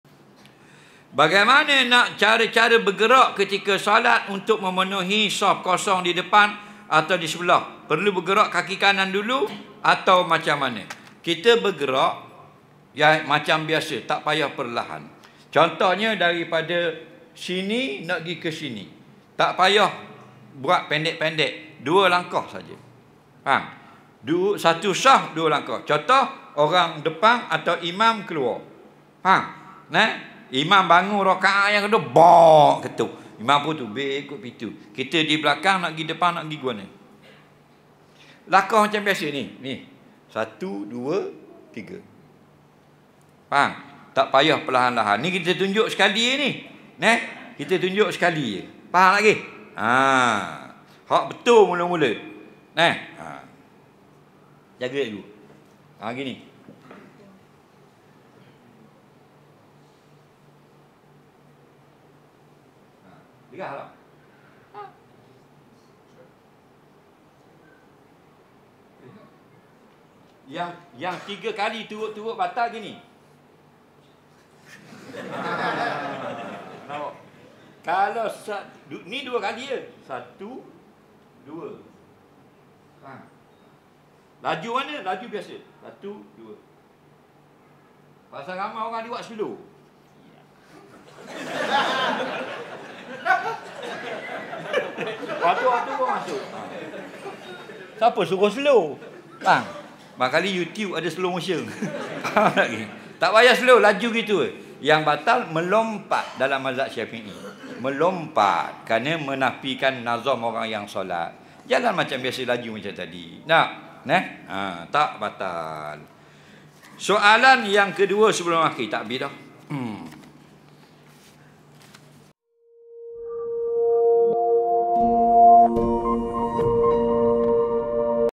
Cara Bergerak Nak Isi Saff Kosong - Ustaz Azhar Idrus
📆 13 Mei 2024 | Isnin
⏰ 7.45 malam
🕌 Masjid Al-Furqan, Simpang 4, Chendering, Kuala Terengganu, Terengganu
📖 Kitab : Tuhfah Al-Raghibin